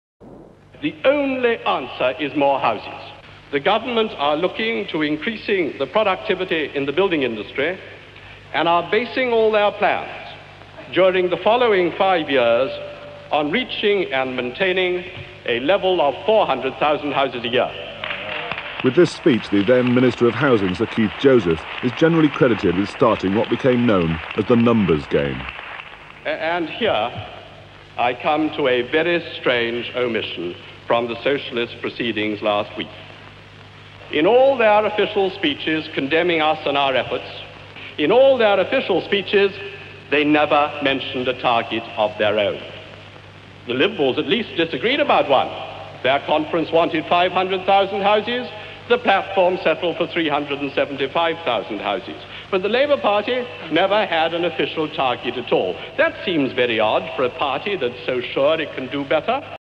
Sir Keith Joseph Speech, 1963
Conservative Party Conference, 1963. Sir Keith Jospeh, then Minister of Housing, speaks.